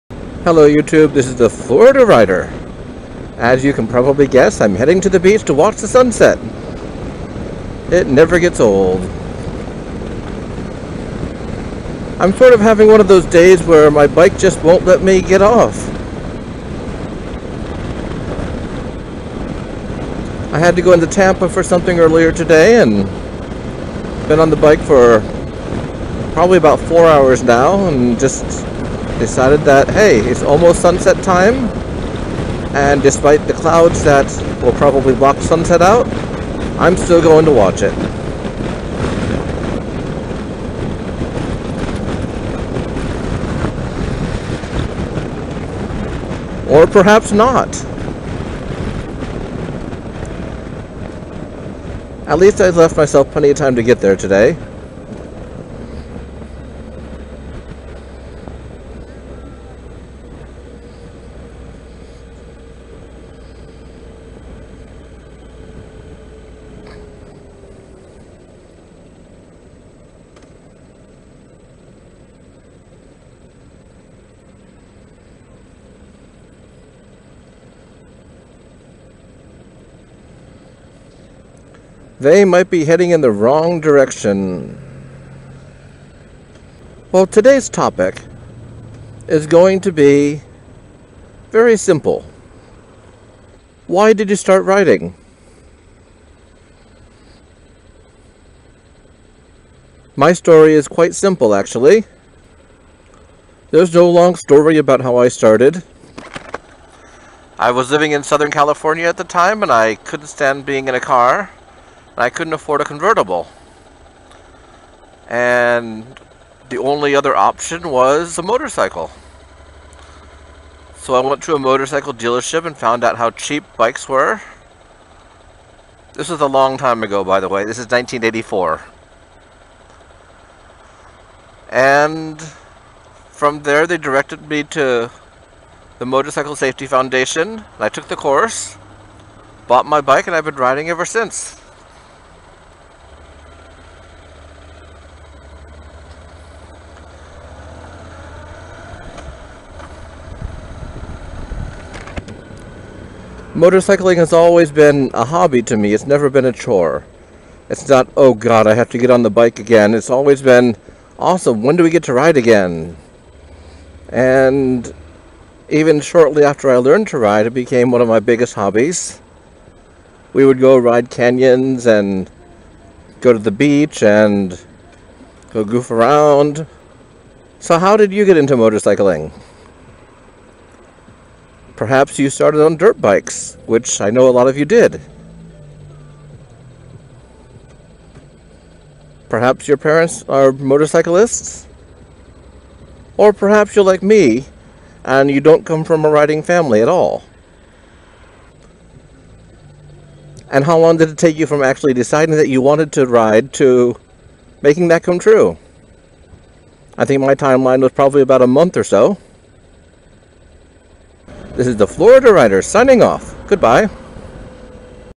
How/Why did you start riding?
Here's my story, what's yours?

Uploaded on Feb 24, 2015